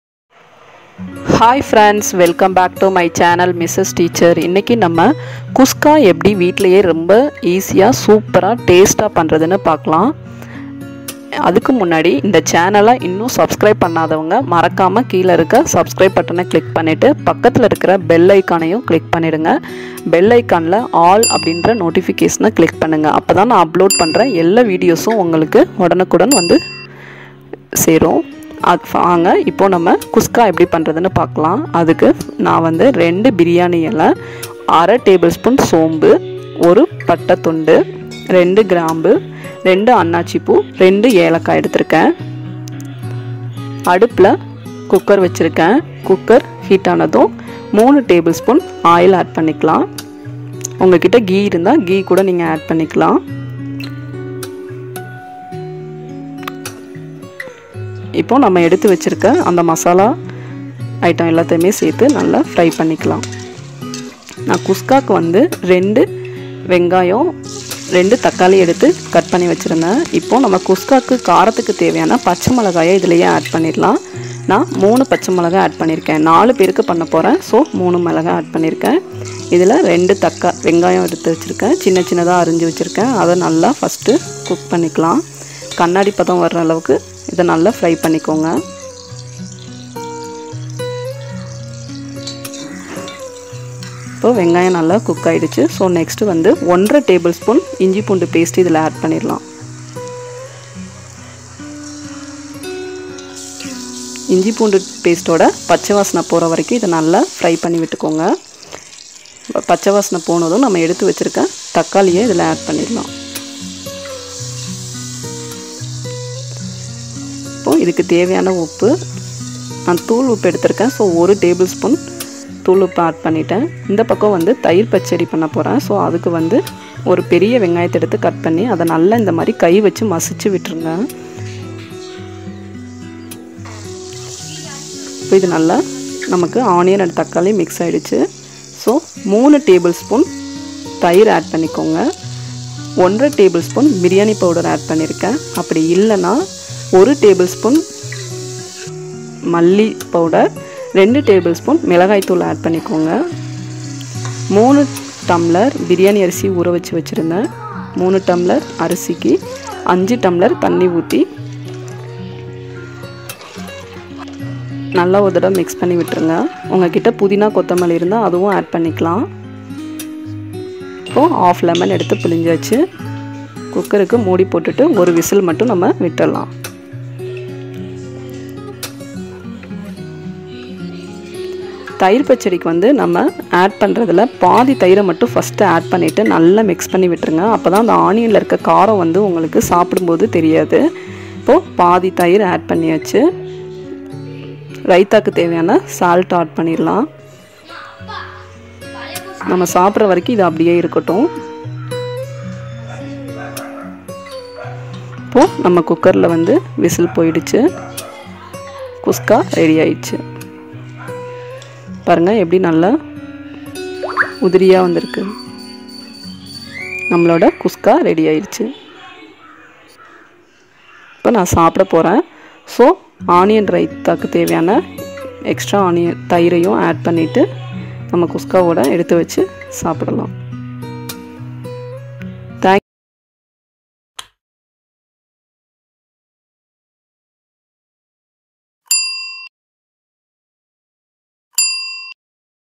How to make Khuska | Khuska recipe in tamil | Mrs Teacher | empty briyani
In this video contains how to make Khuska very simple and tasty.

Ingredients for the Khuska Recipe:-
Cinnamon sticks- 1no
Gloves- 2 nos
jackfruit flower- 2 no
Onion- 2 nos medium size
Ginger Garlic Paste - 1.5 tsp
Tomato - 2 nos Medium size
Curd- 3tsp
Briyani Powder- 1.5 tsp
Briyani rice- 3 cup

Share my videos to your friends and relatives.

N.Gouthami.